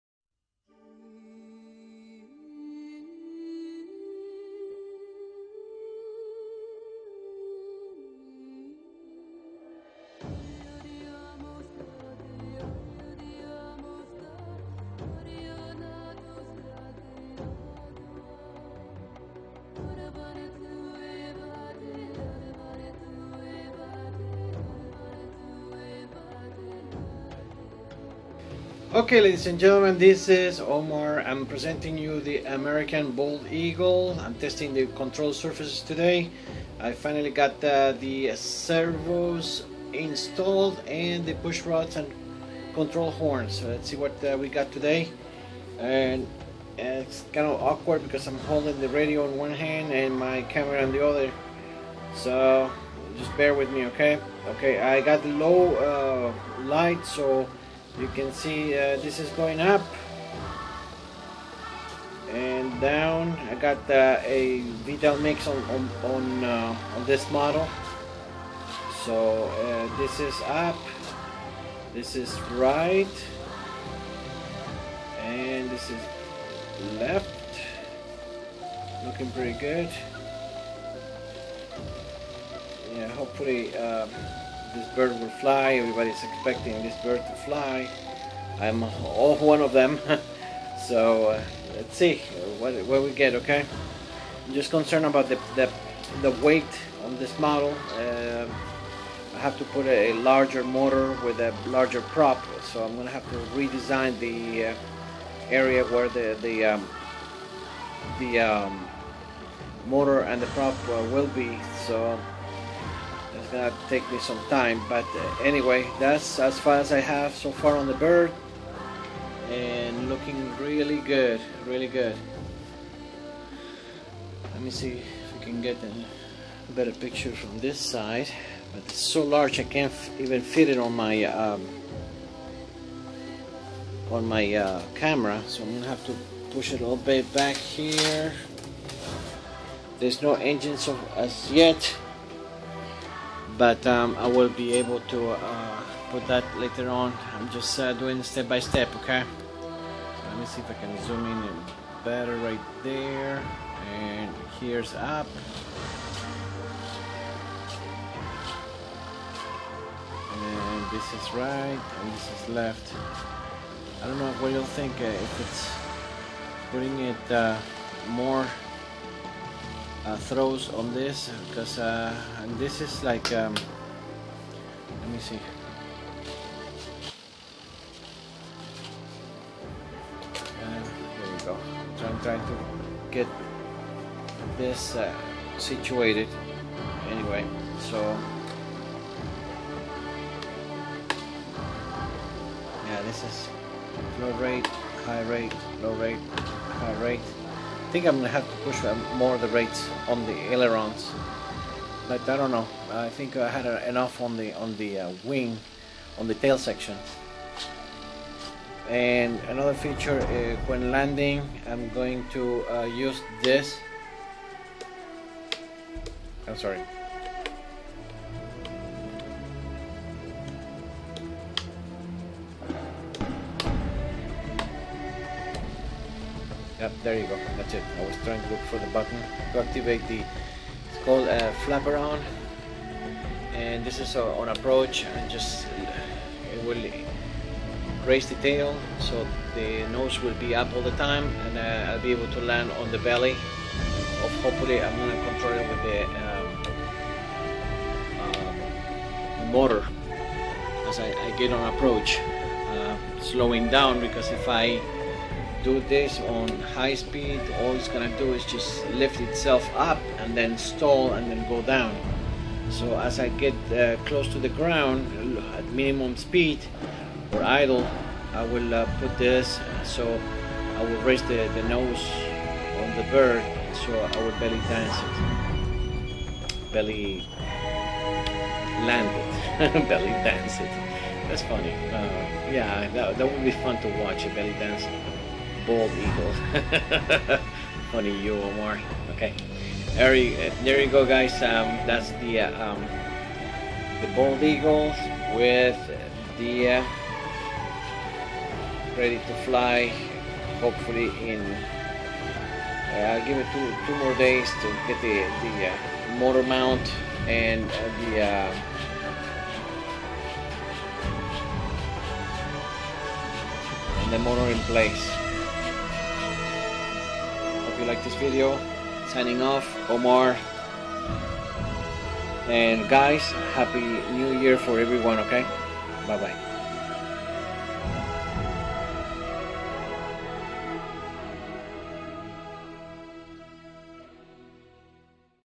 The Americna Bald Eagle C. S. T.
THE AMERICAN BALD EAGLE  RC
Wingspan 70 in, Length 40 in, weight 29 OZ.

CONTROL SURFACE TEST.